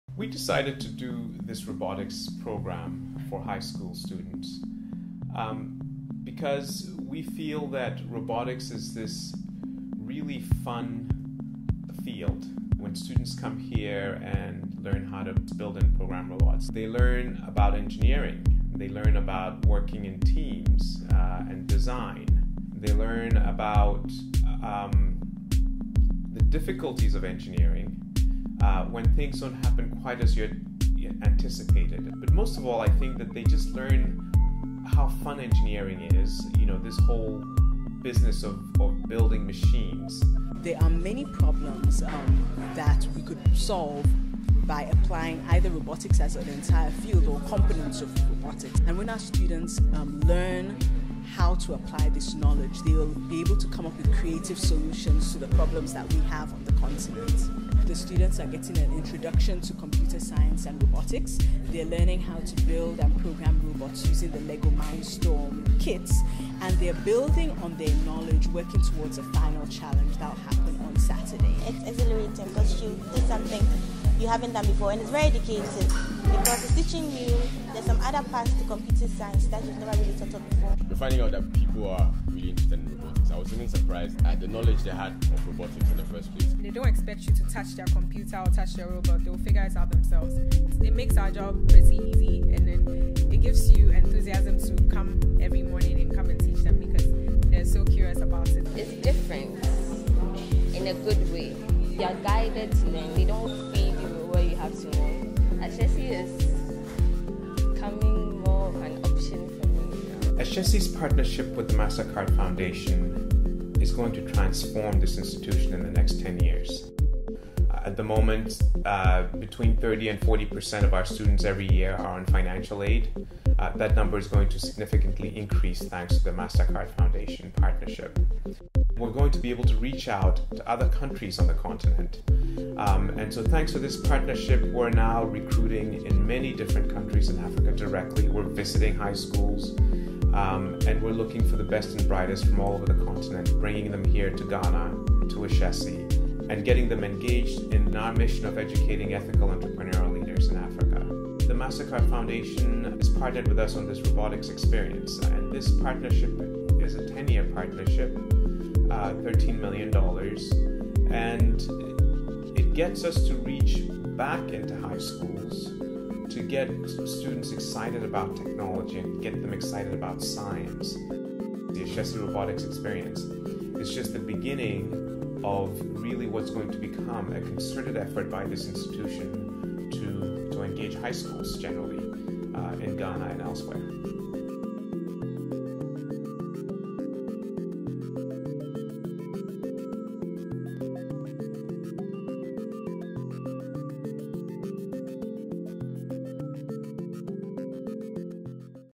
ARX 2012 - A video summary of the first Ashesi Robotics Experience (ARX) programme
On Saturday, August 11, Ashesi announced the winners of our inaugural robotics competition, the Ashesi Robotics Experience (ARX) 2012. The four winning teams were culled from a field of 62 high school students from across Ghana participating in the program.

The program culminated in a competition that challenged students to design task-oriented prototype robots. The competition focused on the mock harvesting of pineapples (which is popular in Berekuso) arranged along a path and in a grid pattern.